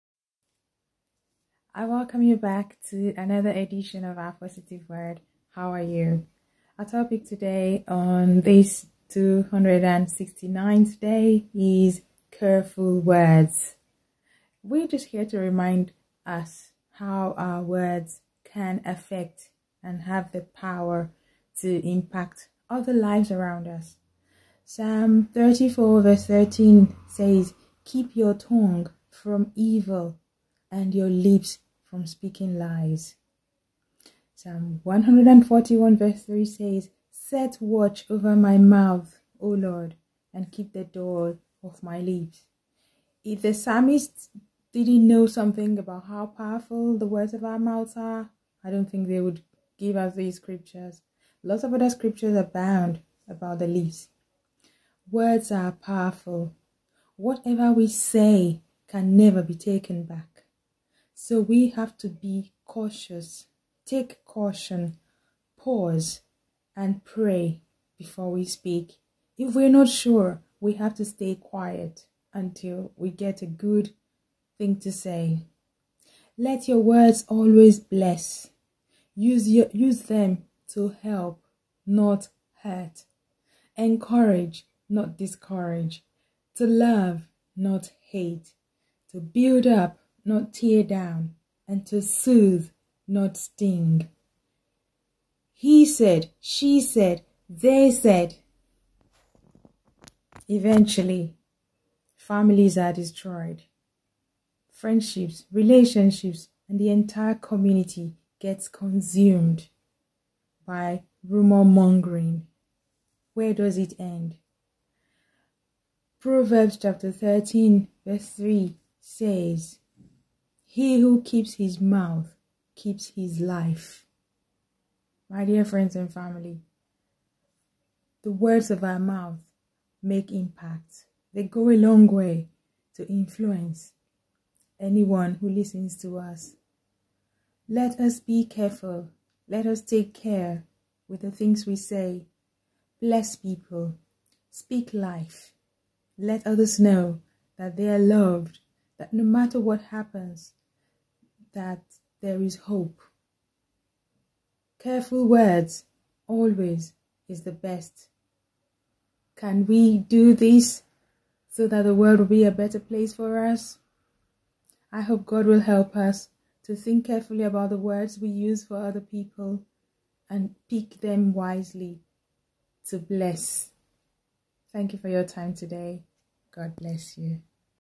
Day 269 Careful Words
Your words have power. Use them to build your life and those of others around you. Speak your dreams into existence.

A truly wise fellow is a man or woman of few words. Take a look around, you will not find one king, queen, important personality who talks too much.

Value yourself by weighing your words before you speak. The saying, 'a great talker is a great liar' is indeed accurate in my experience.

The fewer your words, the more wisdom you display and the less fabrications made against you. Use the words you speak to make positive impact in the world around you.

May God help us to set watch over our mouths and keep the door of our lips. Psalm 141:3 May our words give life, strength, healing, joy, peace, and hope to our hearers. Amen 🙌🏾